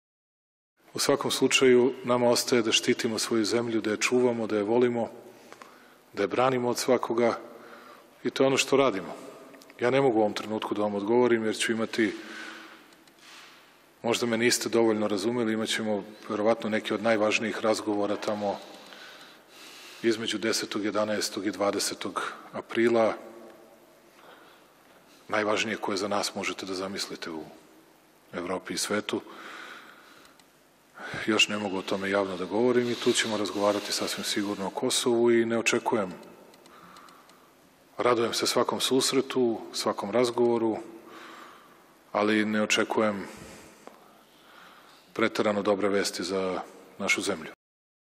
2018.04.03. - Ötmillió euró a Bosznia Szerb Köztársaságnak
Ötmillió euróval támogatja a Bosznia Szerb Köztársaság községeit a szerb kormány - mondta Aleksandar Vučić szerb államfő, miután Milorad Dodikkal, a Boszniai Szerb Köztársaság elnökkel tárgyalt.